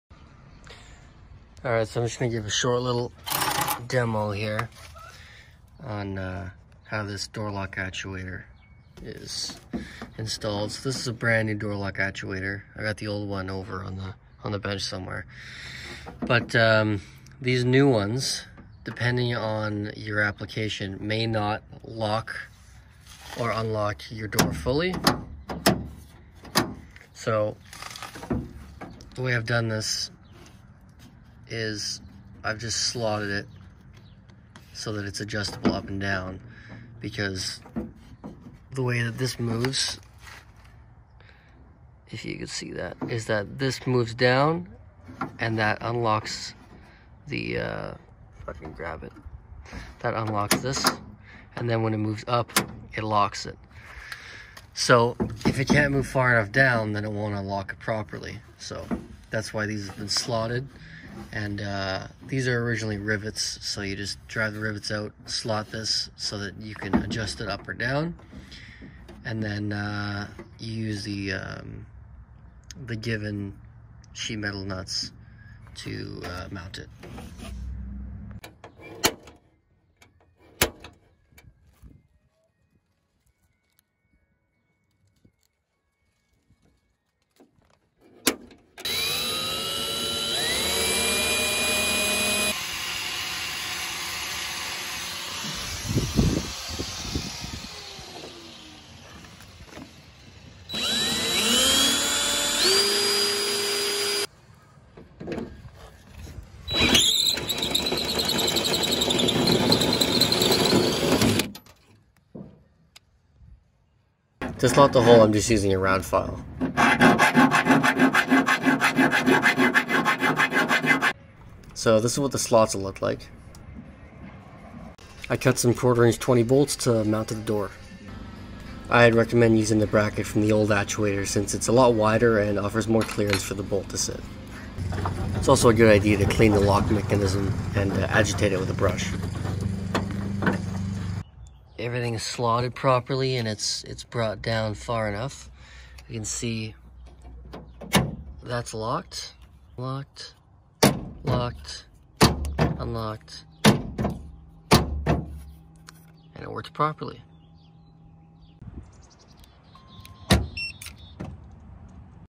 Third Generation F-Body Door Lock Actuator Install
How to install new door lock actuators on a third generation f-body car. The car in this video is a 1988 Chevrolet Camaro.

Replaces GM P/N: 19251546